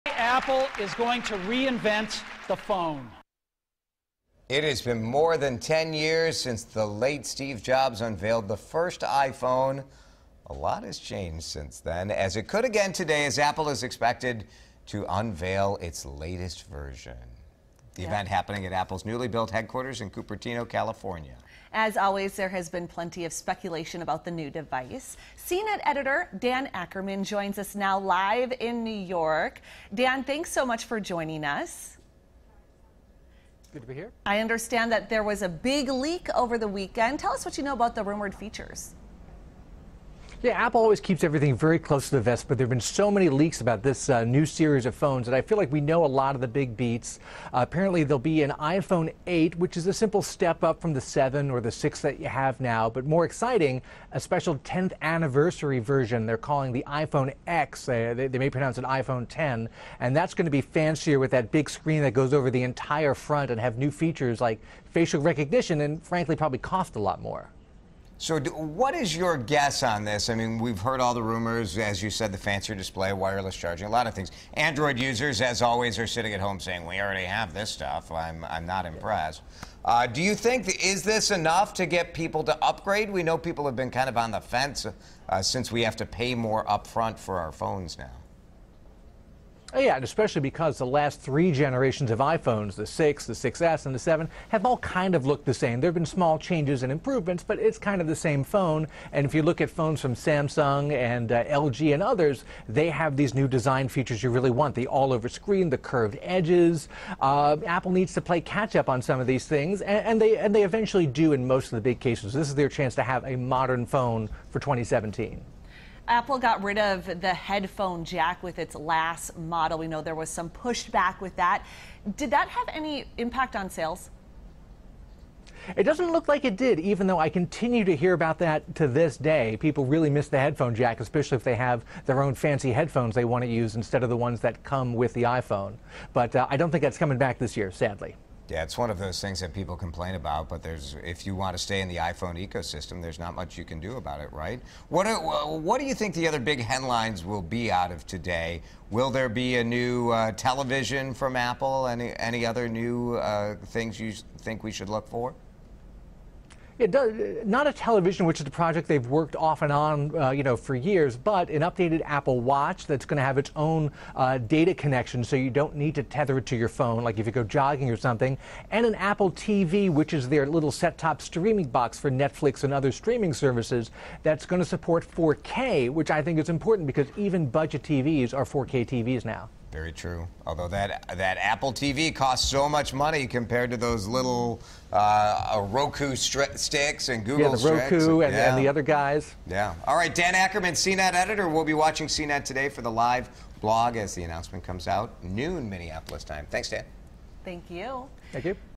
Apple Expected To Unveil New iPhone Tuesday
Apple is expected to unveil a new iPhone today. The company is holding a product event at its new corporate headquarters in California. (3:19)
WCCO This Morning – Sept. 12, 2017